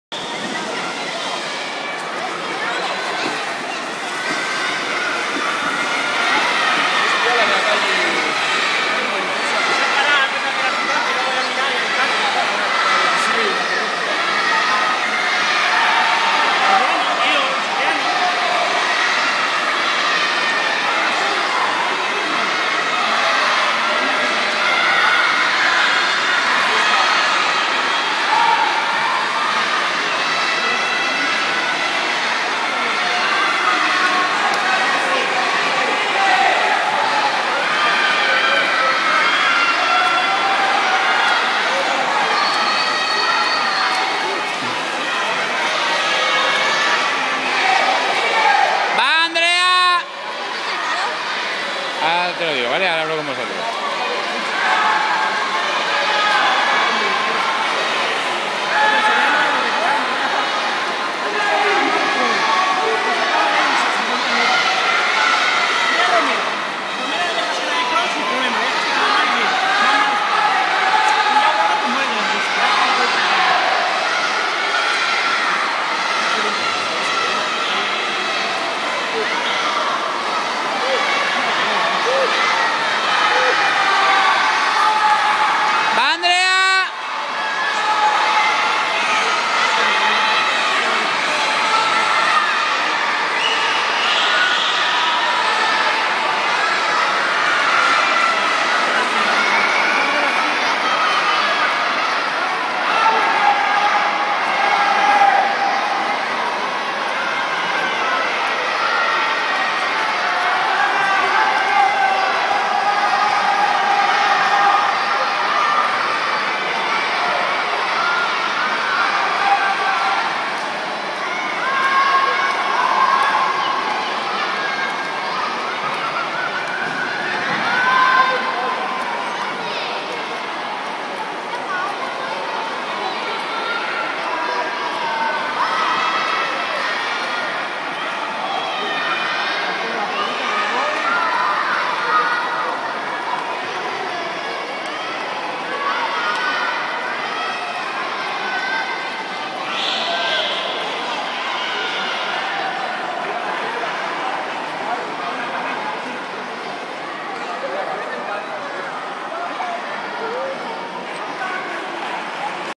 Andrea Sánchez. 100 Estilos. Piscina El Plantío, Burgos.
Prueba 100 Estilos femenino año 2002. Primera jornada Liga Territorial Benjamín Gran Premio Caja España. Piscina El Plantío, Burgos. 25 m.